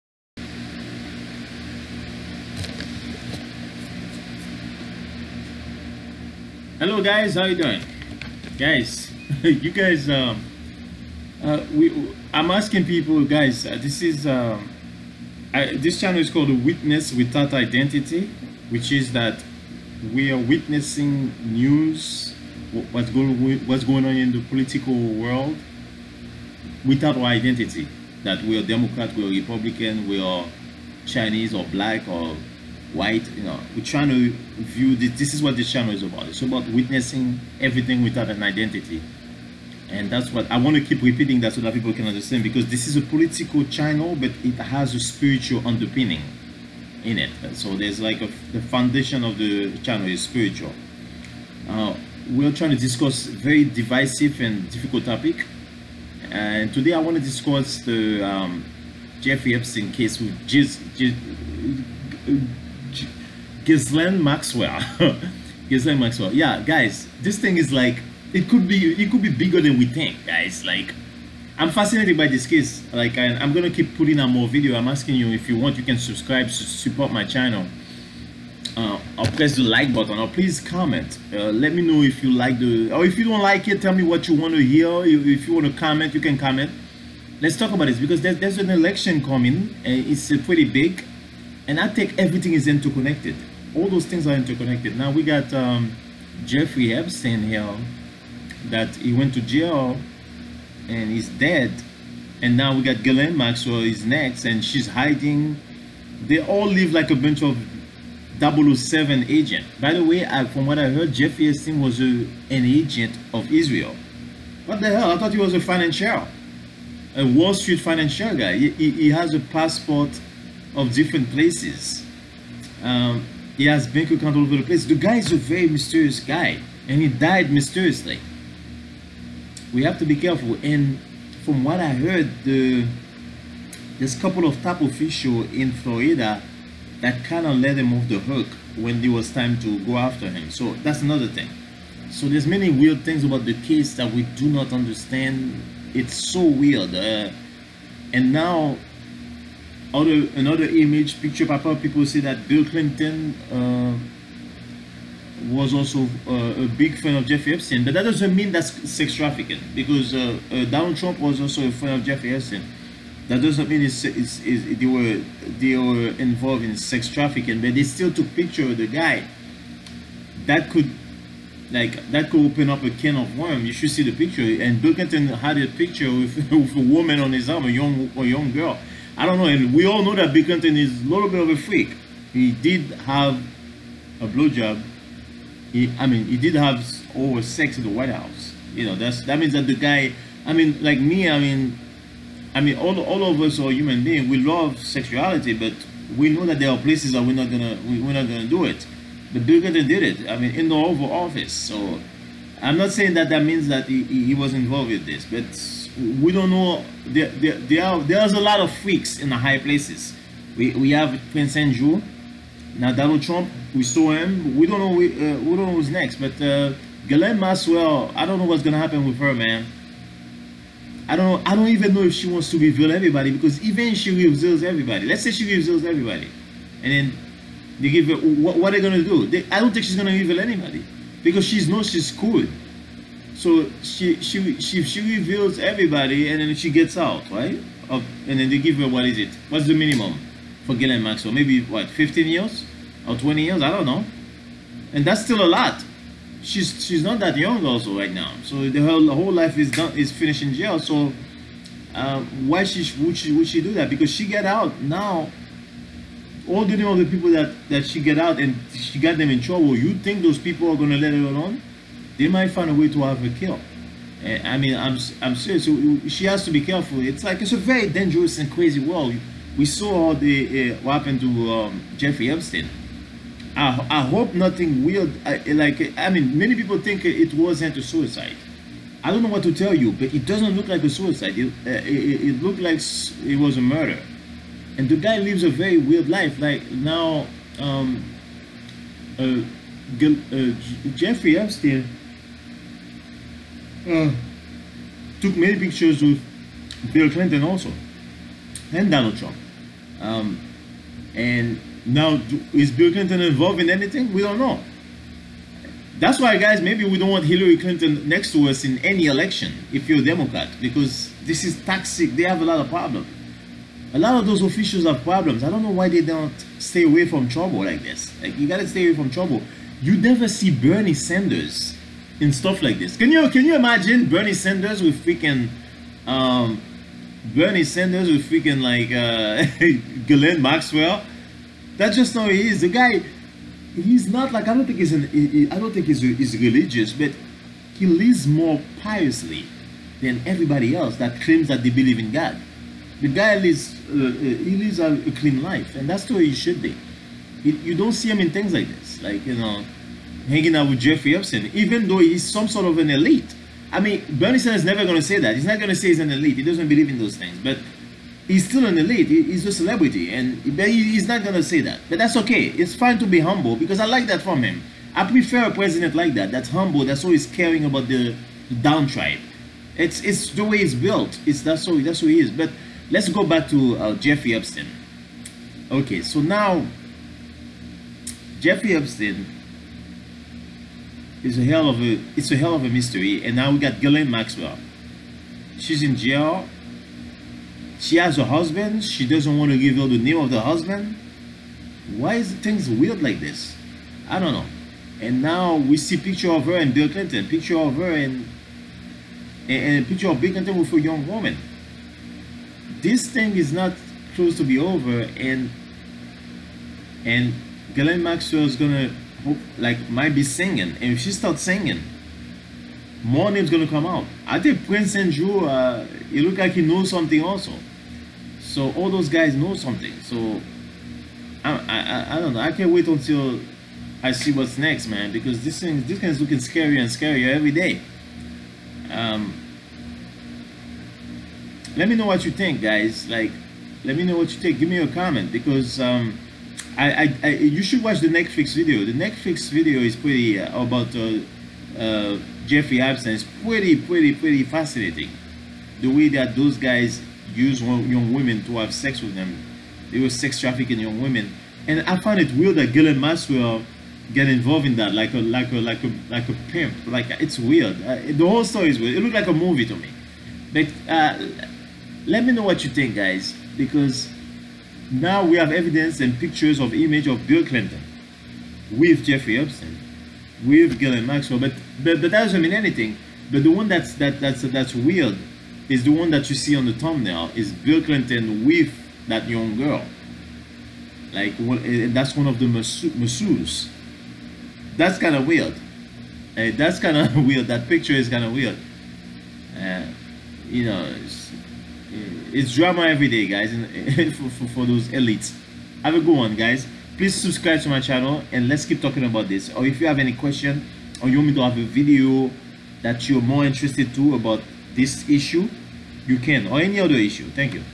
Ghislaine Maxwell and Jeffrey Epstein Hung Out Also With Bill Clinton
Bill Clinton hung out with Ghislaine Maxwell and Jeffrey Epstein. Now  a second person also claimed that Bill Clinton visited the late Jeffrey Epstein’s island in a new Netflix documentary, but the former president again denied these allegations.
Netflix’s Jeffrey Epstein: Filthy Rich,  includes an interview from a longtime tech worker on the Caribbean island who claimed he once saw Clinton at Epstein’s villa home.  In this video I am discussing how the story of Jeffrey Epstein and Ghislaine Maxwell could spiral out of control, with new names of powerful men being released.  Who knows which new name Ghislaine Maxwell will release.